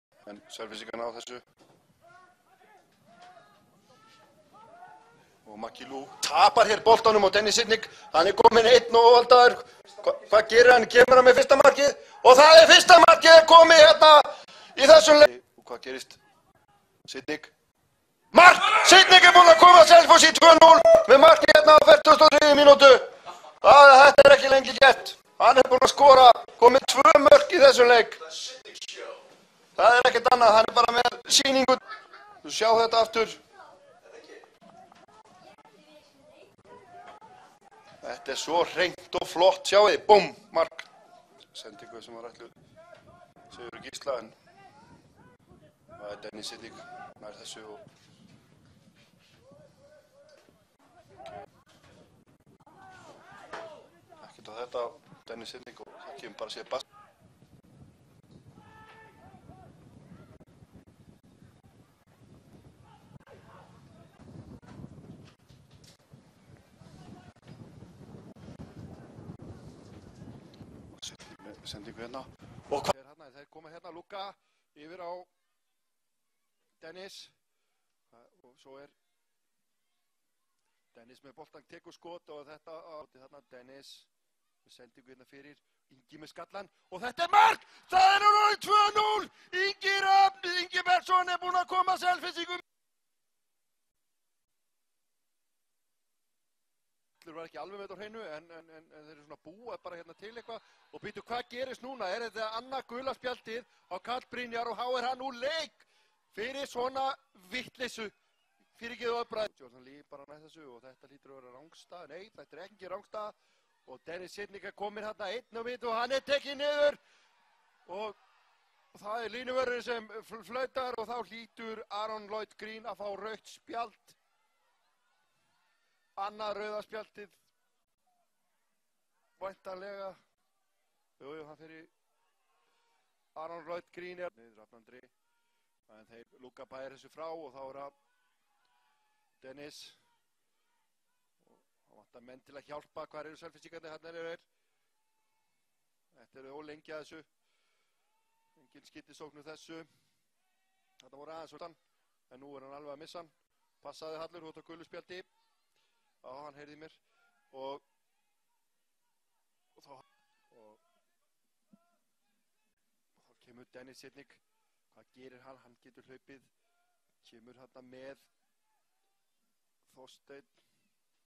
Denis Sytnik. Highlights of two important games. Iceland. Season 2015
The highlights of two very important games for the club Selfoss. Denis Sytnik was an absolute leader on the field with 2 goals against Fram and with an assist, and 2 red cards against the leader of the Championship which brought another very important 3 points for the club.